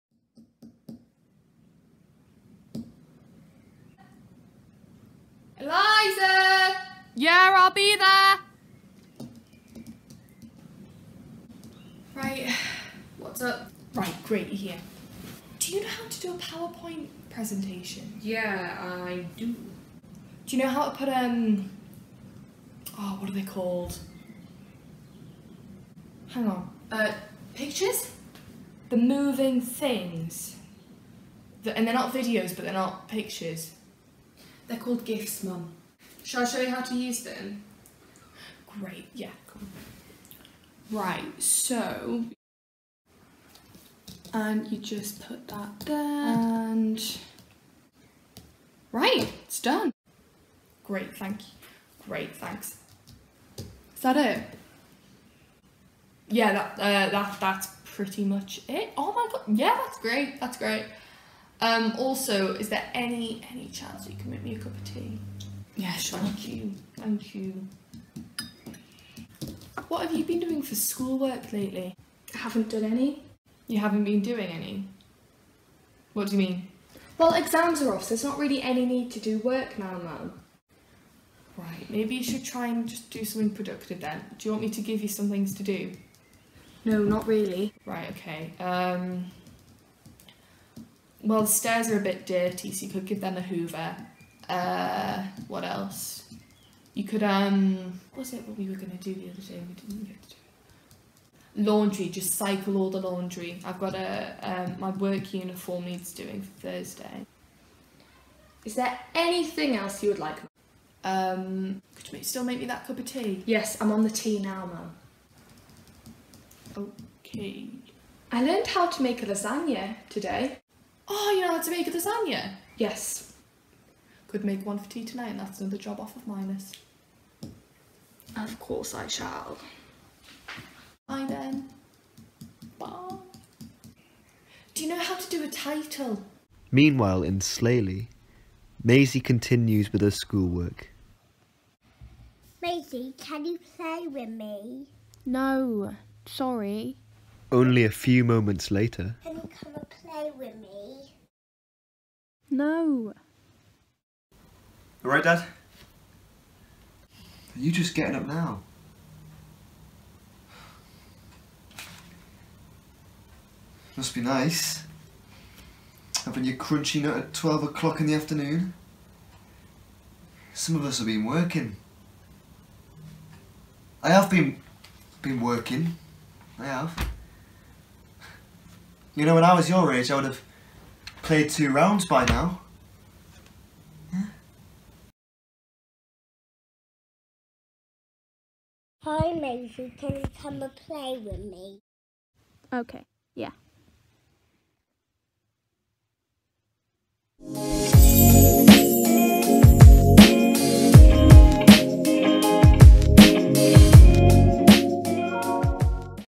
Ensemble - Week 9 - Who We Live With
In Week 9 of Ensemble's CoronavirusTimeCapsule, we get to meet the people our young company members have been spending their mant weeks of lockdown with (well... sort of).

Our young people are brilliant at playing their family members - this week's video is funny, adorable, and the performances are just bang on.

We've only got a few more instalments to go, so make sure to like and subscribe to our YouTube channel to catch the final 2 weeks, and catch up on any you’ve missed so far.